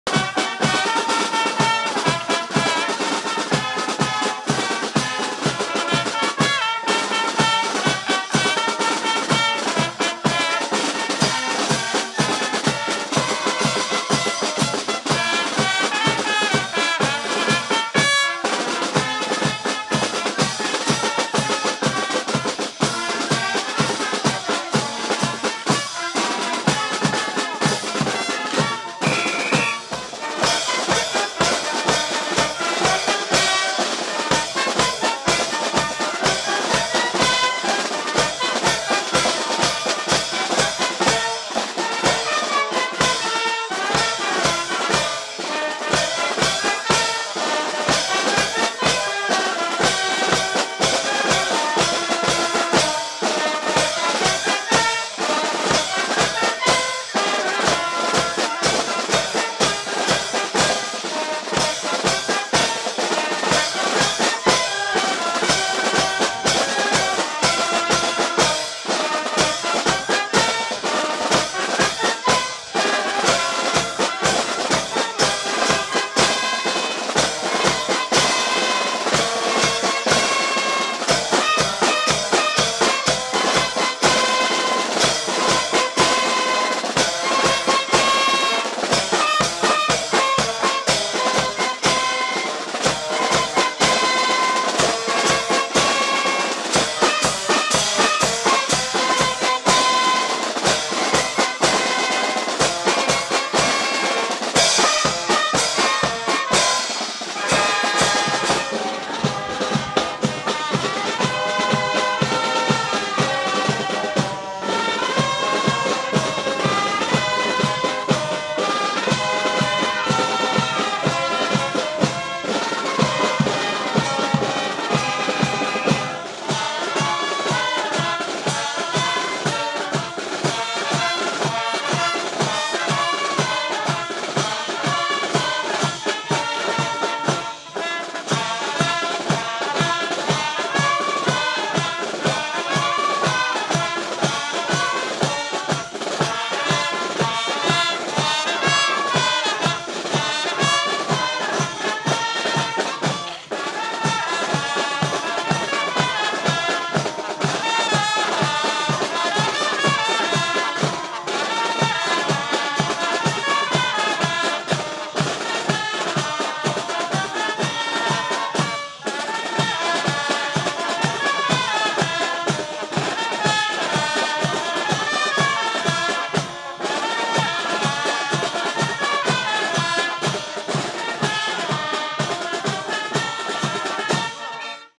Ducasse de Saint-Ursmer avec Dodol et Grisette
Les géants Dodol et Grisette dansent dans les rues du village d'Ormeignies (Ath) portés par les musiques de la Fanfare Royale Union Saint-Denis d'Irchonwelz ce samedi 13 avril 2019